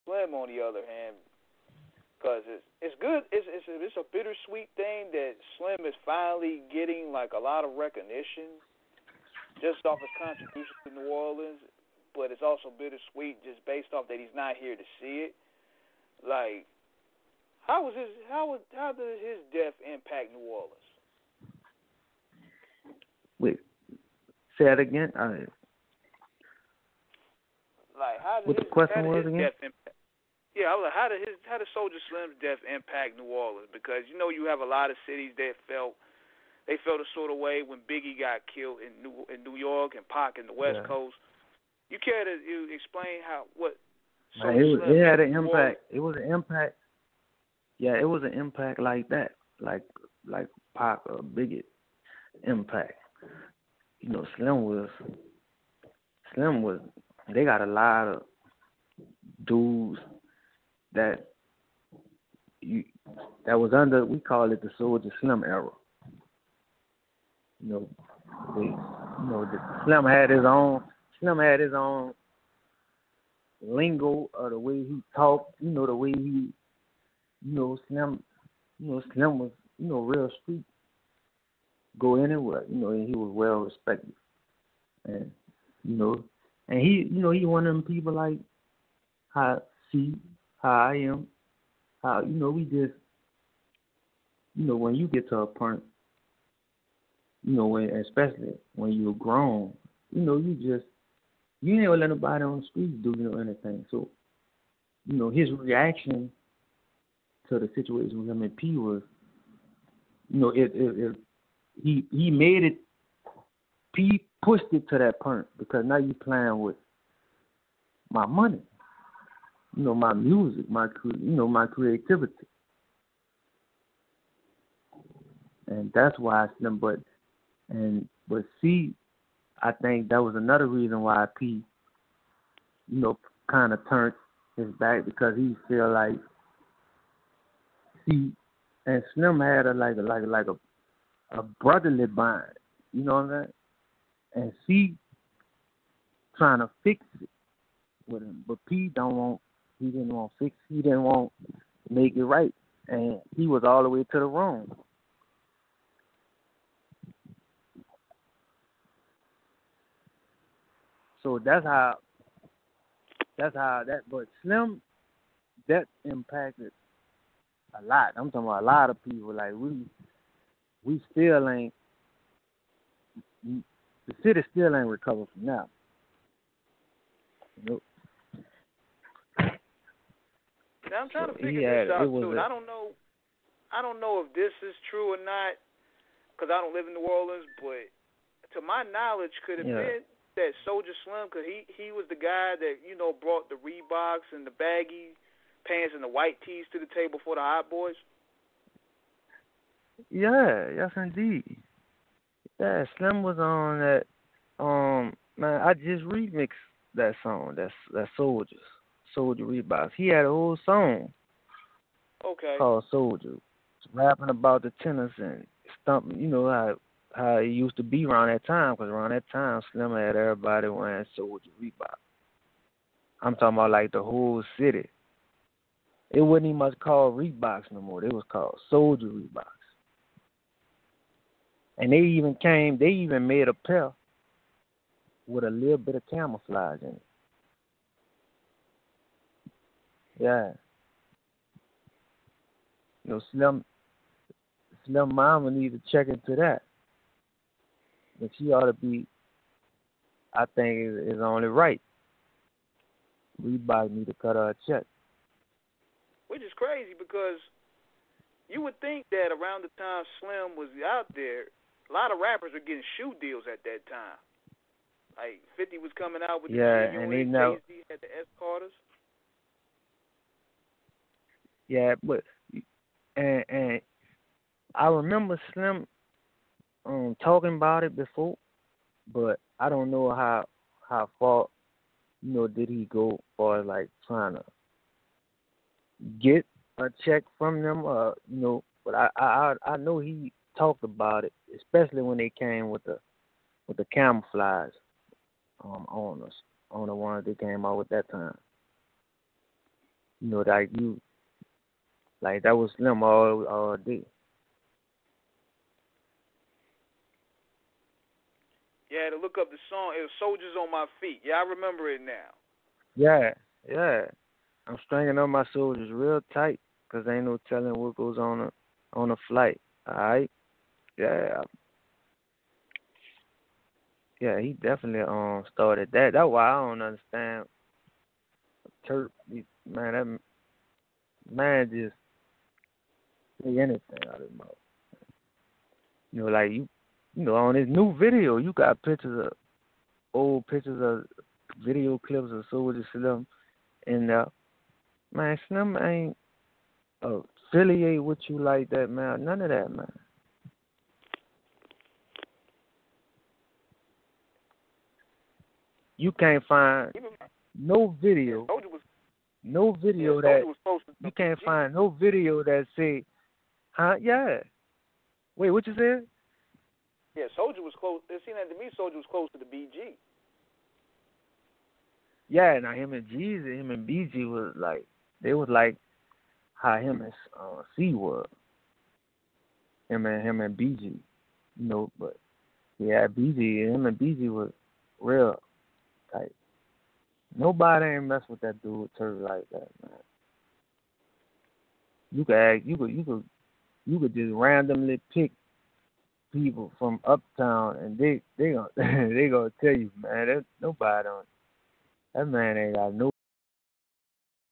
Reginelli speaks on Soulja Slim's influence "He influenced the Hot Boys"
RIP SOULJA SLIM!